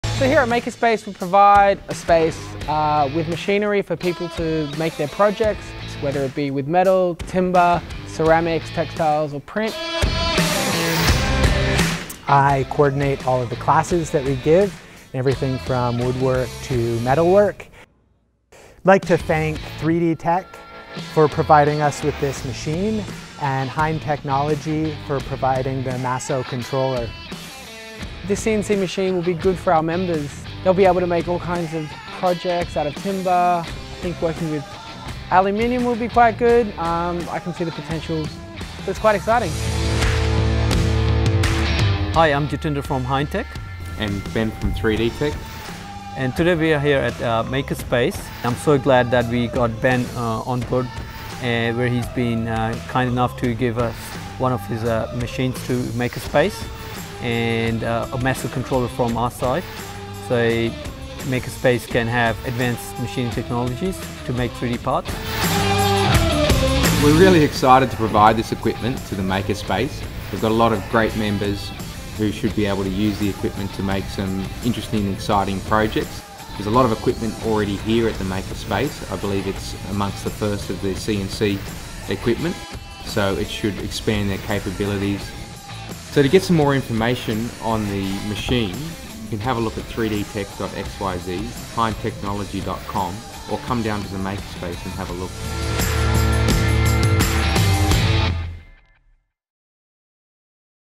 Sharing with the Community at Makerspace
Hind Technology and 3Dtek join hands to provide advanced machining possibilities at MakerSpace in Marrickville, NSW.

The most cost-effective all-in-one CNC Machine Controller that has a fully integrated hardware and software to run Mill, Lathe, Router, and Plasma machines and "DOES NOT REQUIRE A PC" industrial grade.

The MASSO share the same hardware for all the different machine types which makes it easy and simple to wire and configure.